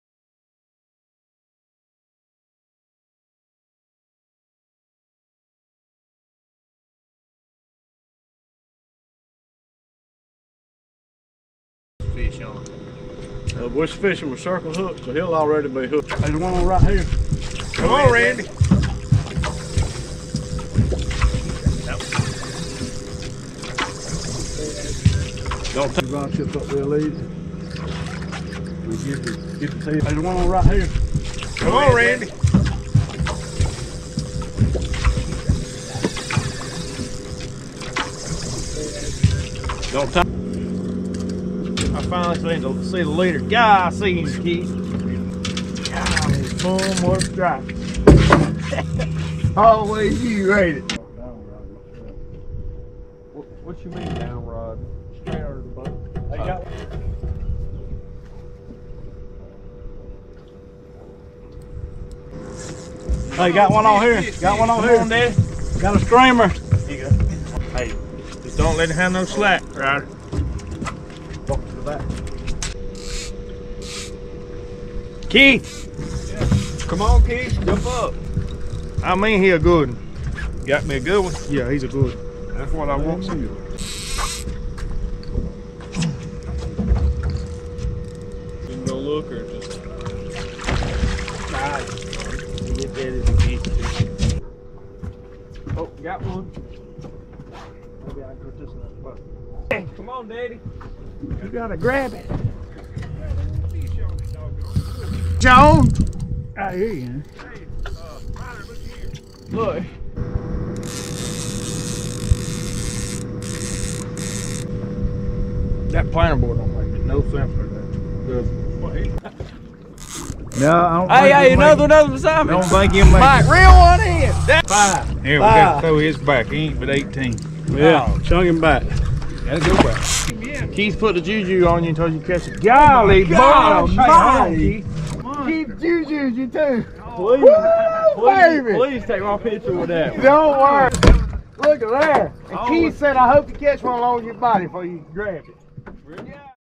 Sunrise Stripers! Lake Murray SC. Live Bait
Fishing live herring for Stripers on Lake Murray SC.  Spring time fast action!